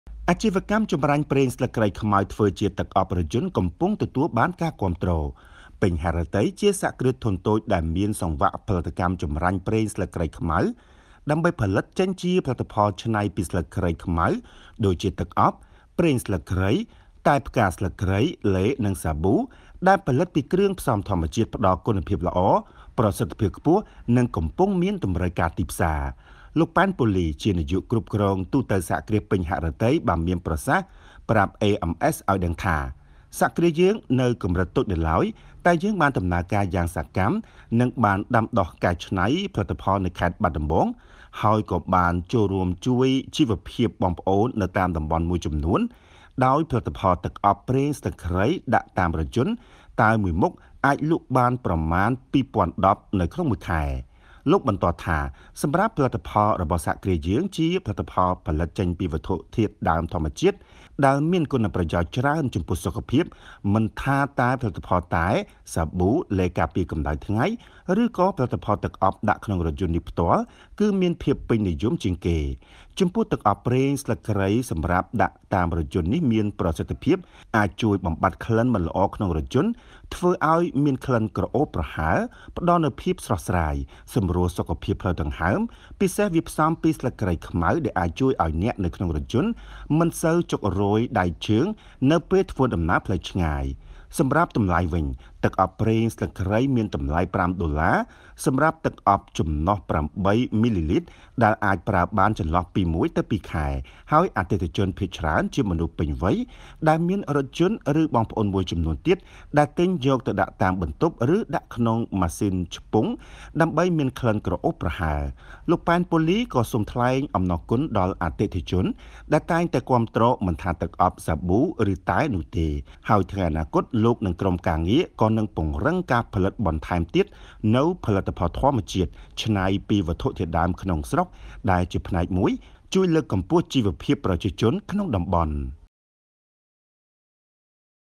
អាជីវកម្មចម្រាញ់ប្រេងស្លឹកគ្រៃខ្មៅធ្វើជាទឹកអប់រថយន្តកំពុងទទួលបានការគាំទ្រ
ស្លឹកគ្រៃខ្មៅ ជាប្រភេទស្លឹកគ្រៃម្យ៉ាងដែលមានក្លិនឈ្ងុយជាងស្លឹកគ្រៃធម្មតា ហើយត្រូវបានគេយកទៅចម្រាញ់ និងកែច្នៃជាផលិតផលជាច្រើនដែលល្អ និងមានប្រយោជន៍ចំពោះសុខភាព។ ក្នុងនោះ ស្លឹកគ្រៃខ្មៅក៏ត្រូវបានគេច្នៃជាទឹកអប់សម្រាប់ដាក់ក្នុងរថយន្តមានក្លិនក្រអូបឈ្ងុយ ជួយបំបាត់ក្លិនមិនល្អ កំពុងទទួលបានការគាំទ្រ និងមានចំណែកទីផ្សារគួរឱ្យកត់សម្គាល់។

រាយការណ៍ដោយ ៖ សារ៉ាត់
អត្ថបទ ៖ កញ្ញា ម៉ាន់ ហ៊ុនលាង